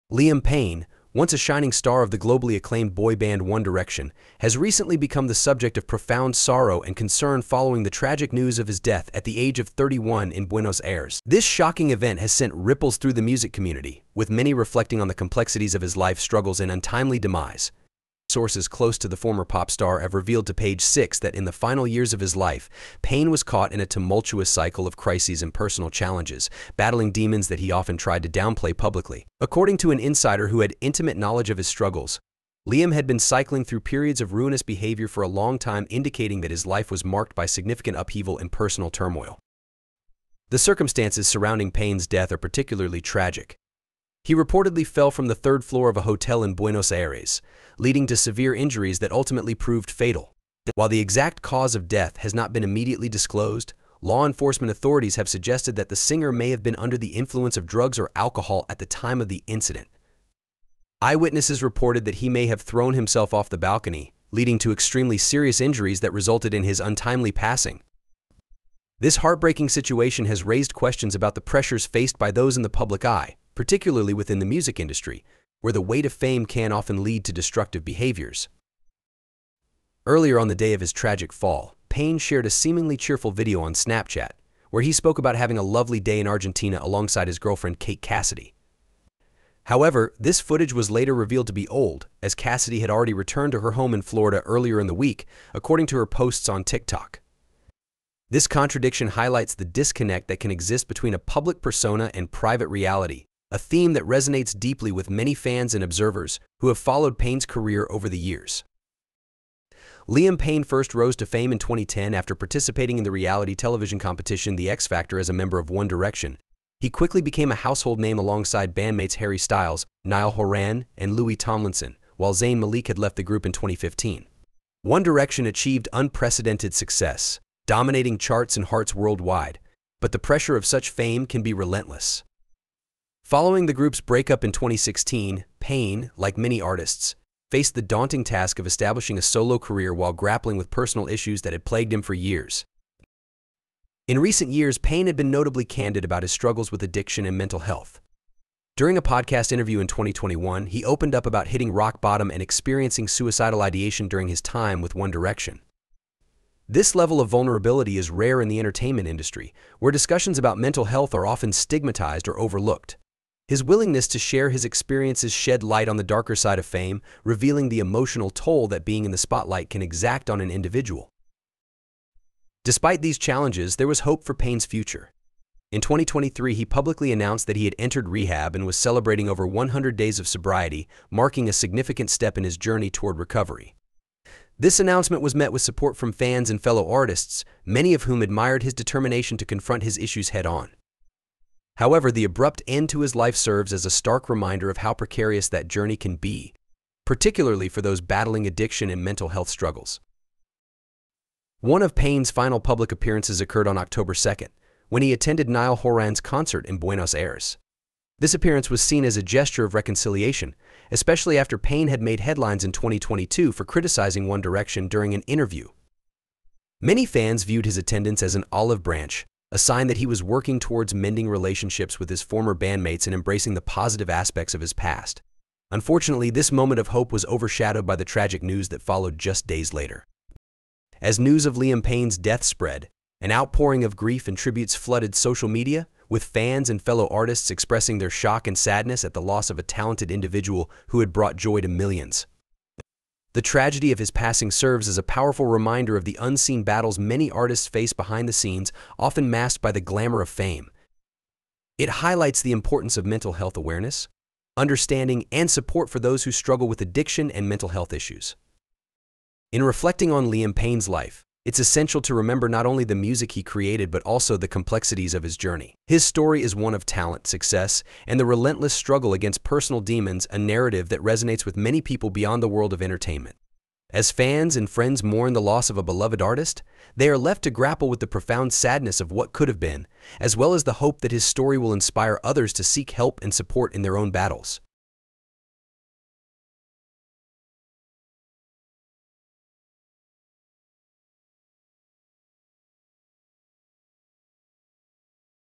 Liam Payne: A Troubled Star’s Final Days and Tragic Passing in Buenos Aires
Discover the heartbreaking story of Liam Payne, the former One Direction member who faced significant struggles with mental health and addiction in the final years of his life. Following his tragic death at 31 in Buenos Aires, details emerge about his battles and the pressures of fame. From candid interviews about his experiences with suicidal ideation to his journey toward sobriety, learn about the complexities of Payne’s life and the impact of his untimely passing on fans and the music industry.